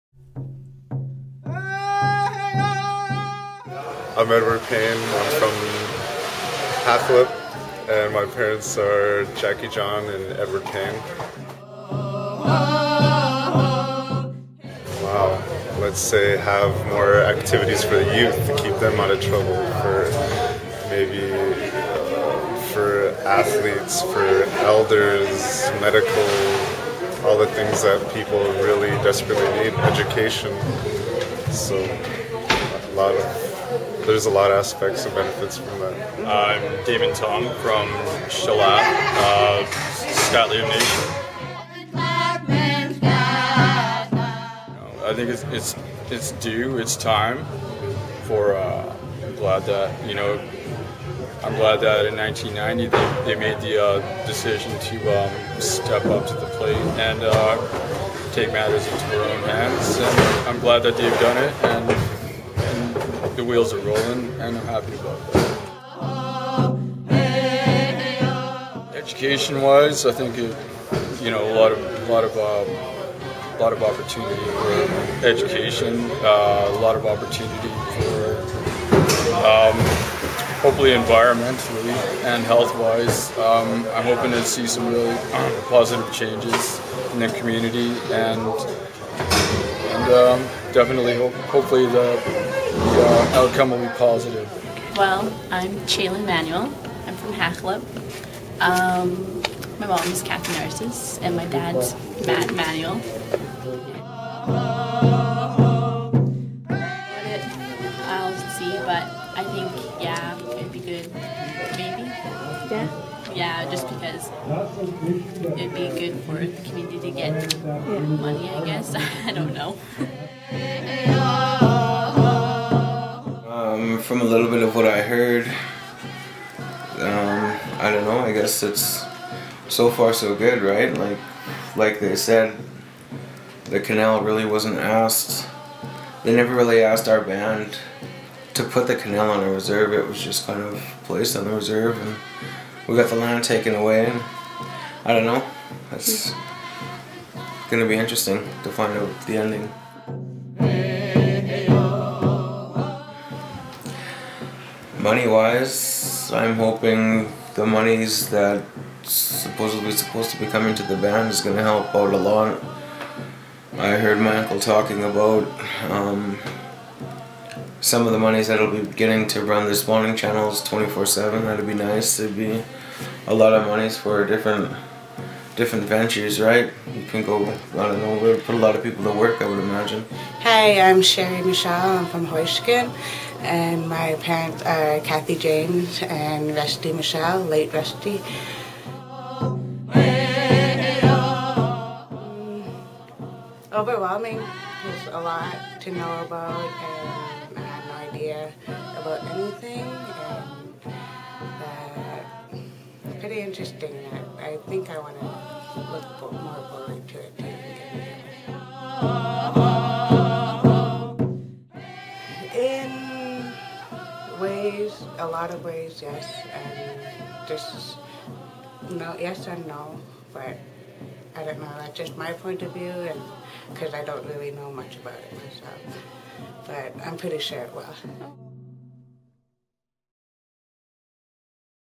St'at'imc Youth
Interviews with the St'at'imc Youth from different communities. They express their thoughts on the agreement, and how the agreement may benefit their communities.